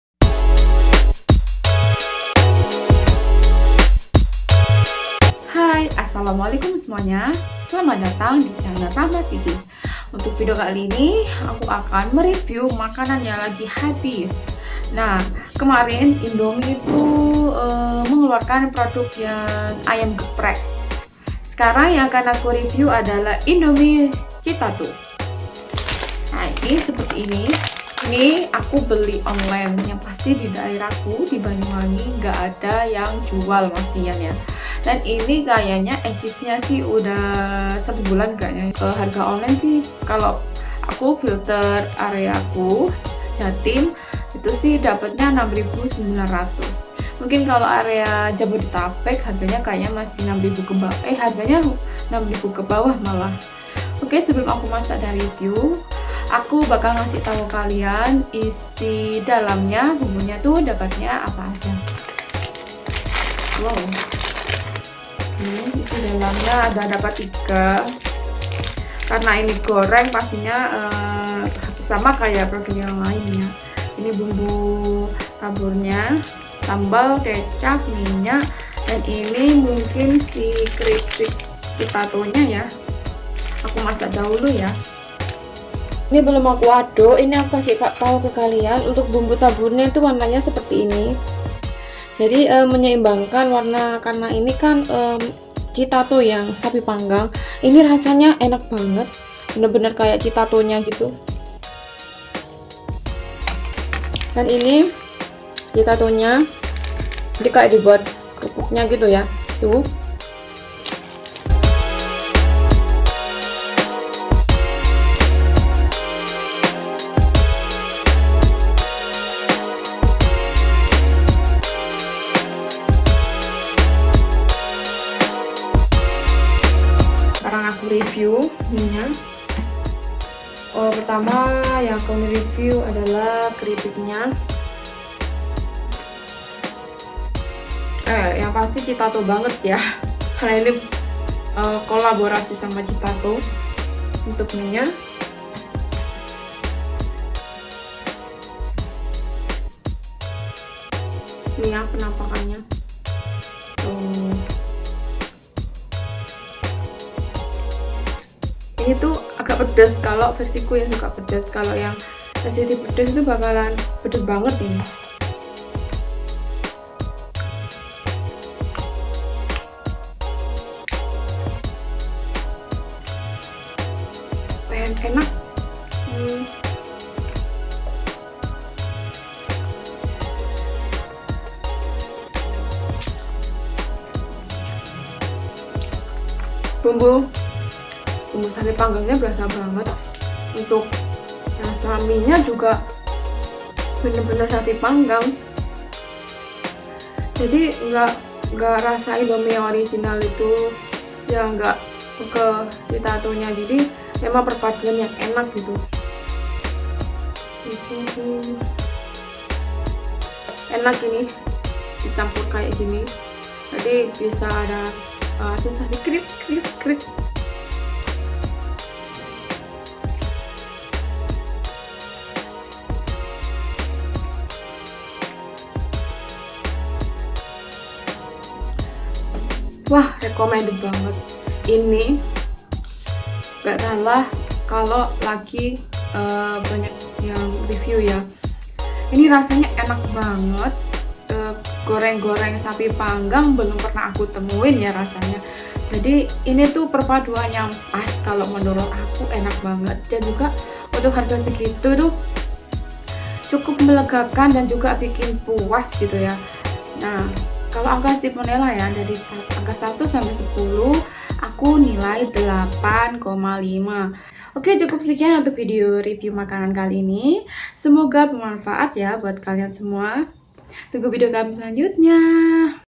Review Indomie Goreng Chitato Sapi Panggang
Perpaduan Indomie Dan Chitato Rasa Sapi Panggang, nikmat dan bikin ketagihan, gak nyesel deh buat nyobain